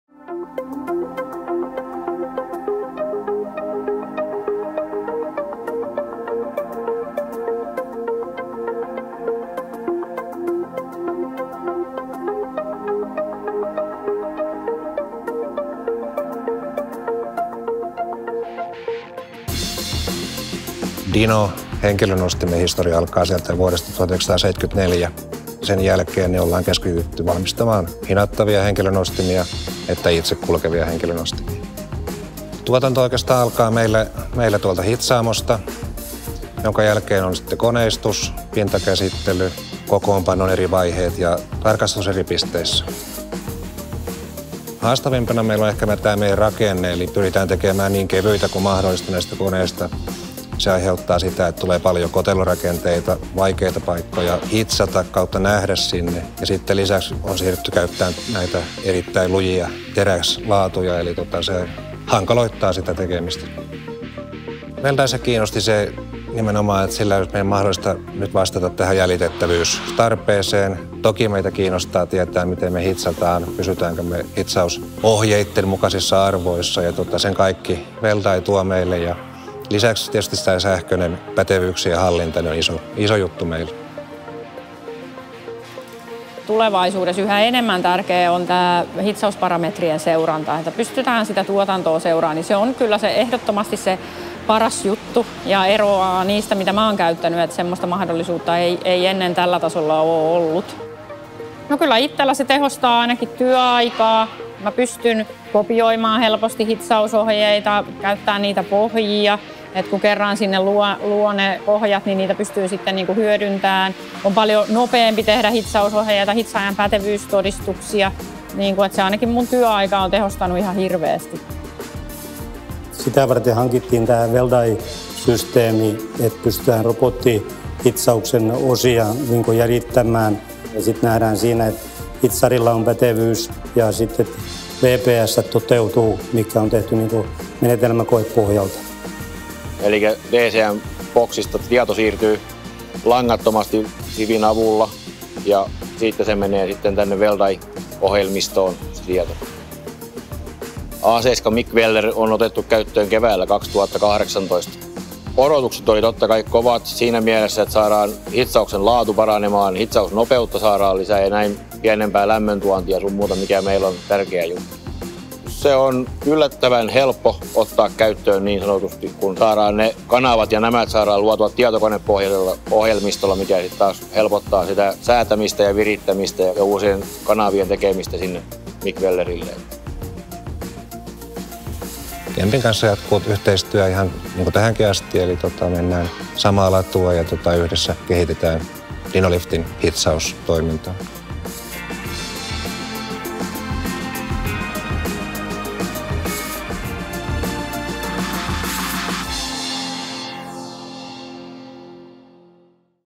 Kemppi reference: Dinolift, Finland - WeldEye, A7 MIG Welder
Dinolift designs and manufactures mobile elevating work platforms (MEWP) that must comply with the requirements of both EN ISO 3834 and AWS welding standards. Powered by Kemppi's A7 MIG Welder power sources, Dinolift's robotic welding cells are integrated with WeldEye welding management software, which ensures that the MEWPs are welded according to the welding procedure specifications. The software also provides 100% traceability in robotic welding.